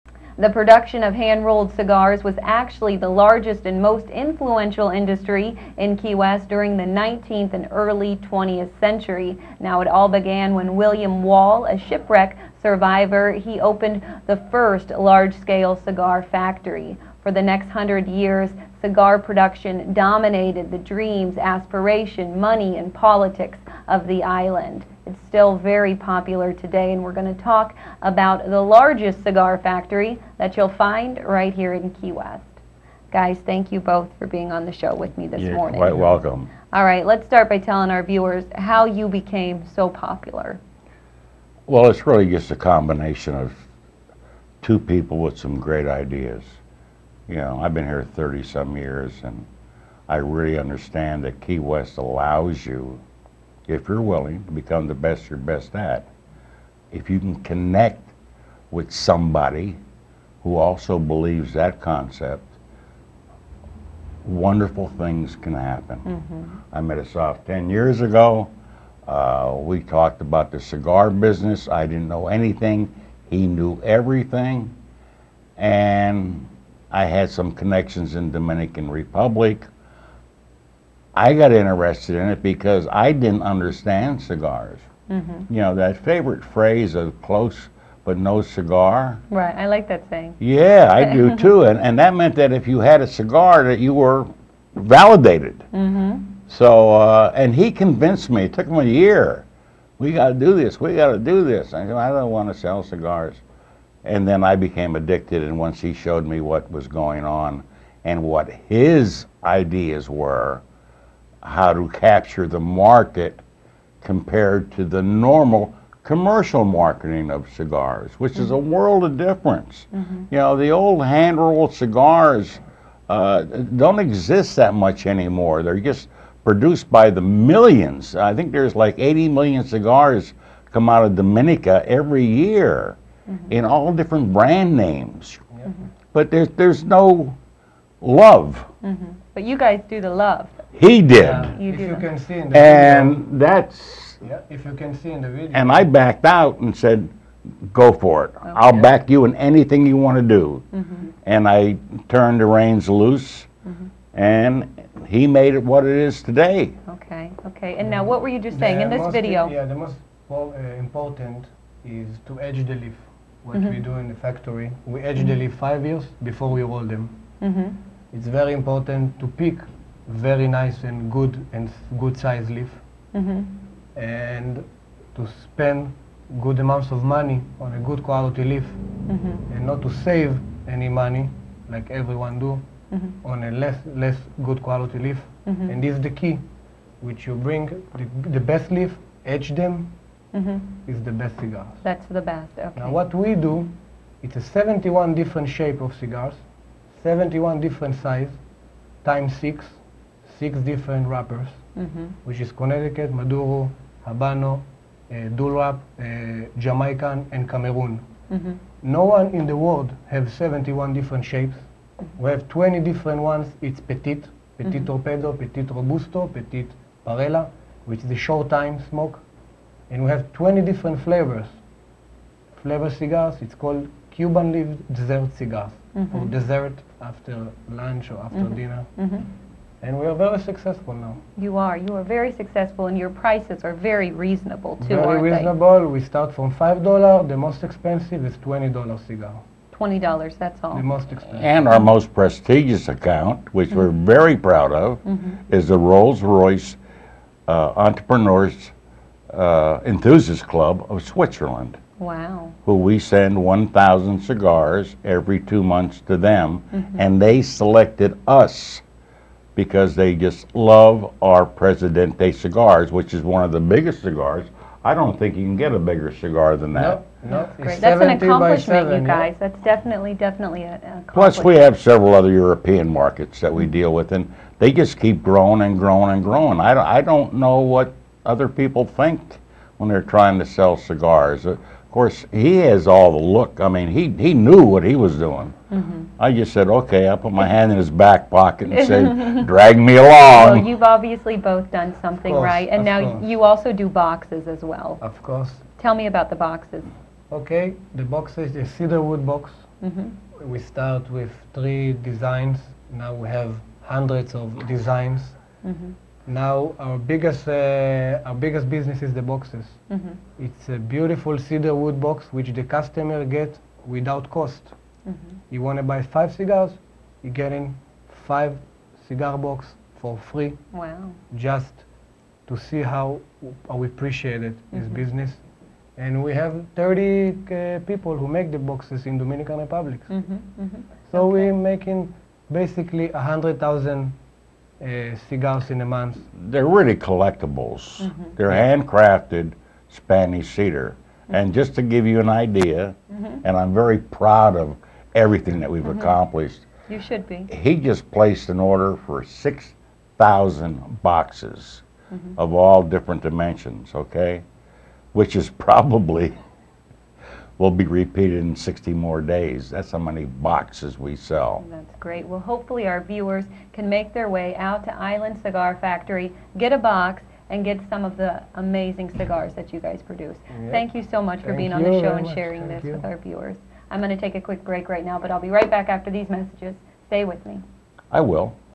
Good Morning Florida Keys with Jenna Stauffer Clay Greagar and Assaf Azoulay Is. Cigar Factory
Clay Greagar and Assaf Azoulay from Island Cigar Factory talk cigars!